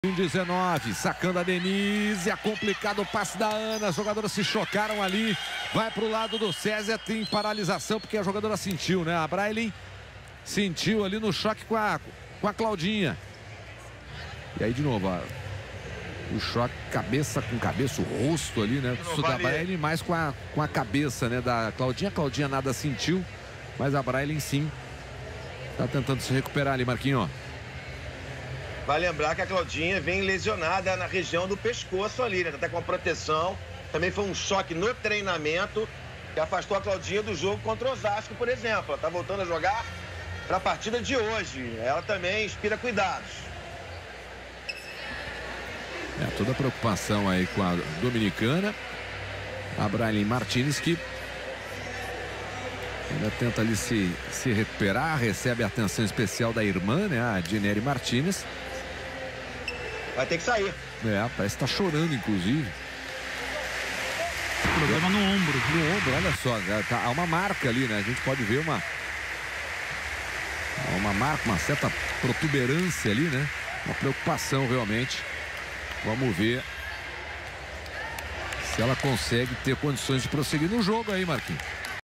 MOMENTO DE TENSÃO| Martínez se lesiona no segundo set e chorando precisa ser substituída
A oposta levou a pior ao tentar recuperar a mesma bola que a levantadora Claudinha. Acompanhe! ￼